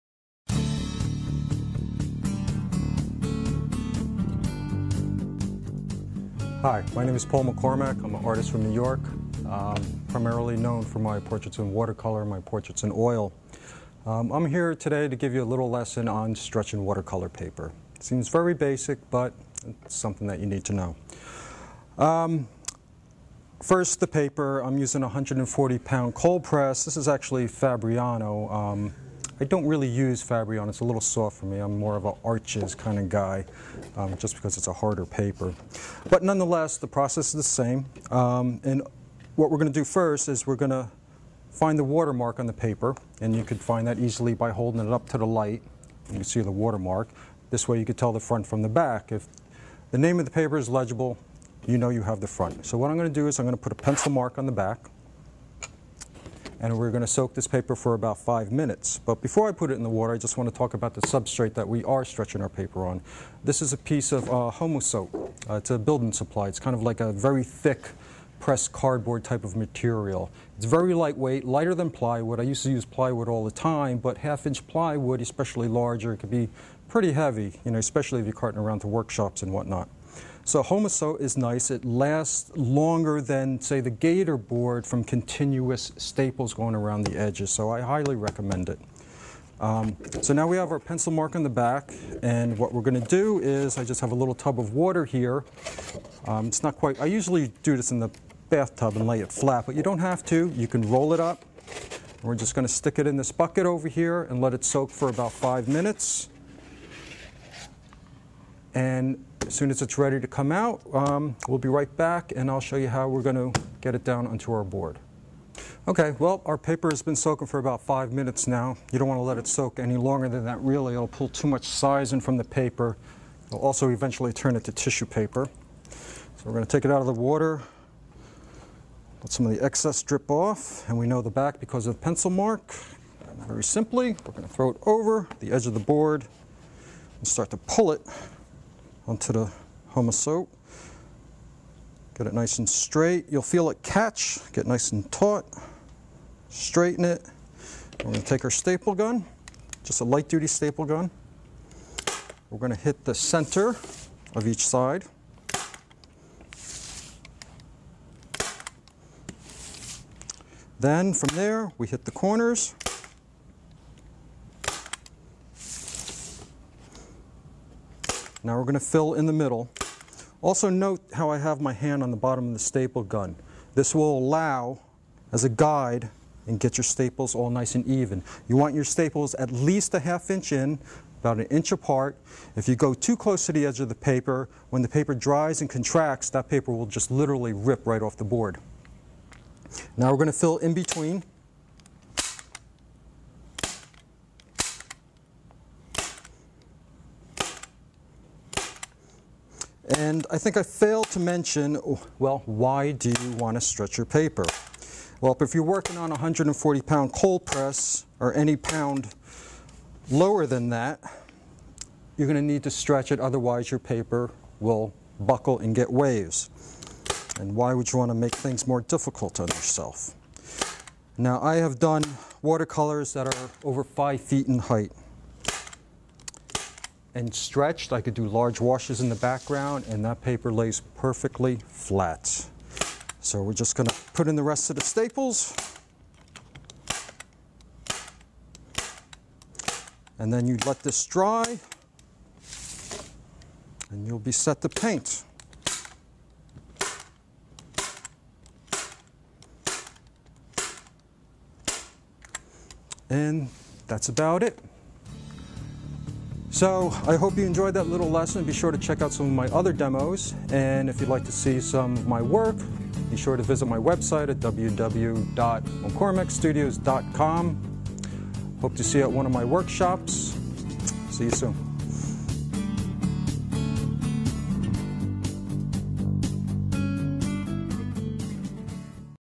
How to stretch watercolor paper
Filmed for Jerry’s Artarama in 2011, Paul McCormack shows you how to stretch watercolor paper.